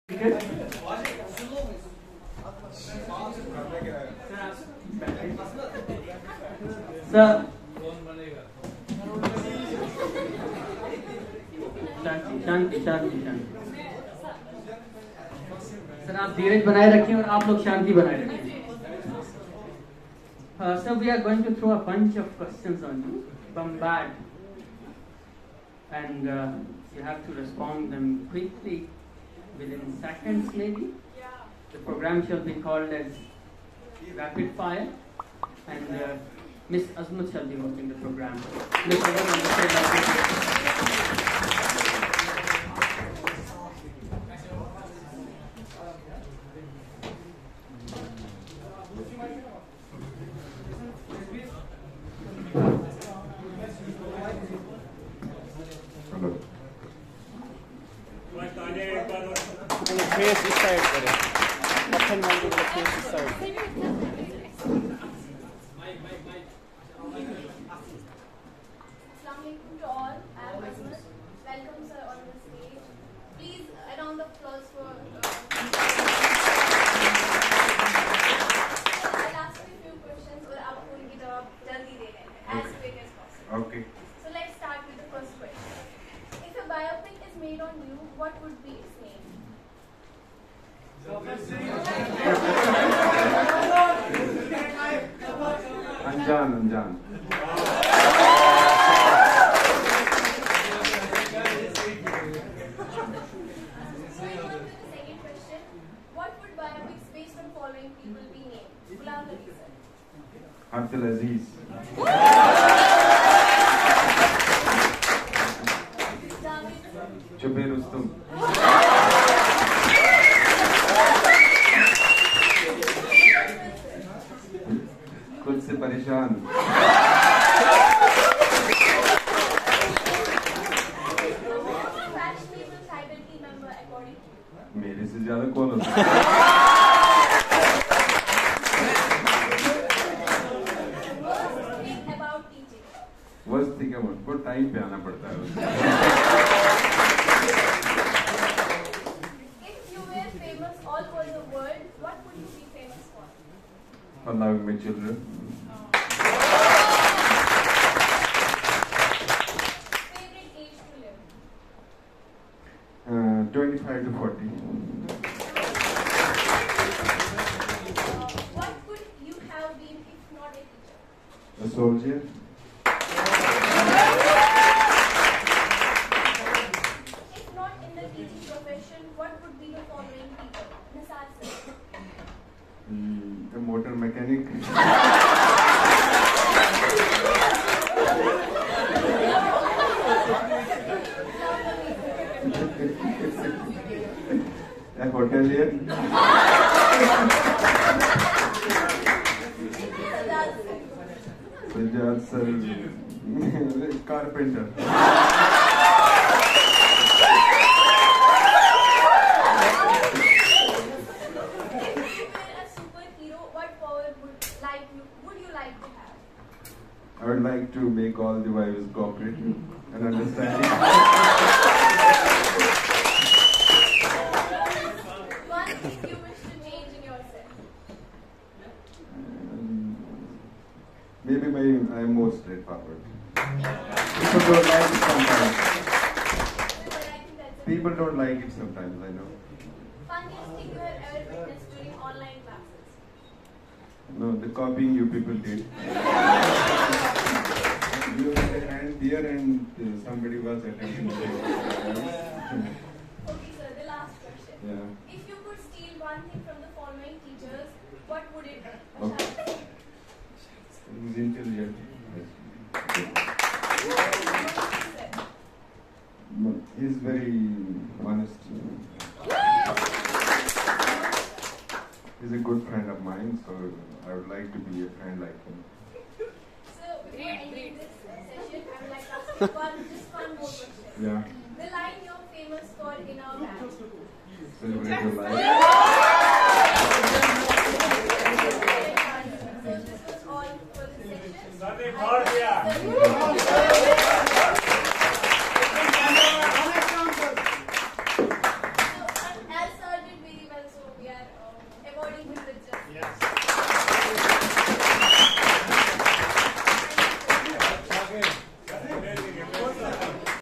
Rapid Fire with prof Waseem Bari | Kashmir University Department of physics
Rapid fire session with professor Waseem bari function at department of physics University of Kashmir.

Keep sharing and show some love for bari sir

Kashmir University department of physics